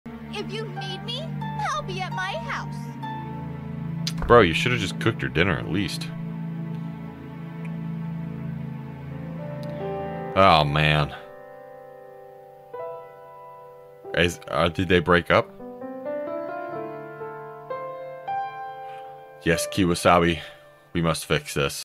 WE MUST FIX THIS!
Things are looking a little BLEAK! We better fix this Spider style. Just let me grab my bowtie.

Join RatherSlack as we play matchmaker and get this relationship back on Track! We just may be small... And a spider.

Full Video on the Main Channel!